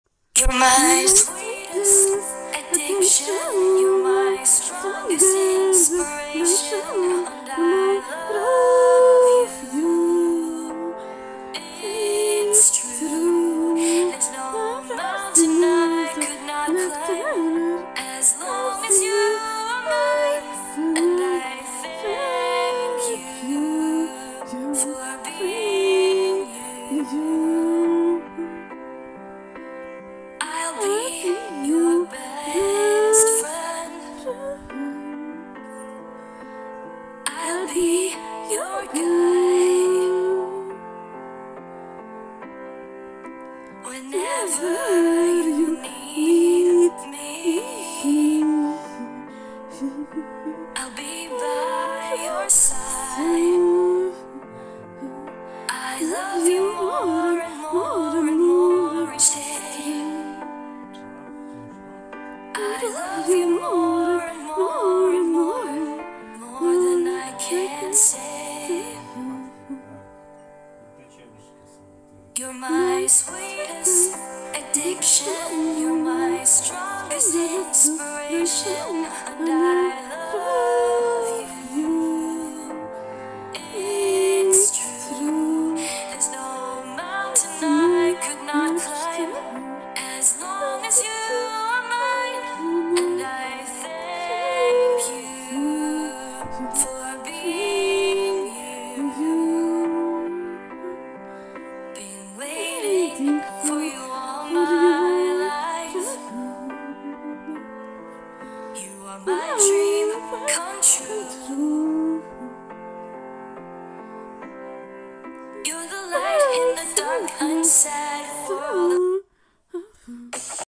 nie wiem jak wyglądasz więc zrobiłem ciebie pszypadkową postać jakikolwiek wygląd❤️dla chłopaka🏳️‍🌈
śpiewam zrobiłem historie gacha life dla mojego ukochanego występuje oczywiście chłopak ja Peter Jezus i Bóg! wierzę w Boga! Nie wiem jak wyglądasz więc zrobiłem ciebie pszypadkową postać jakikolwiek wygląd! A z tymi urodzinami że urodziny masz po prostu mi taki pomysł przyszedł do głowy myślę że historia ci się podoba jest prosto z mojego serduszka dla ciebie! ;)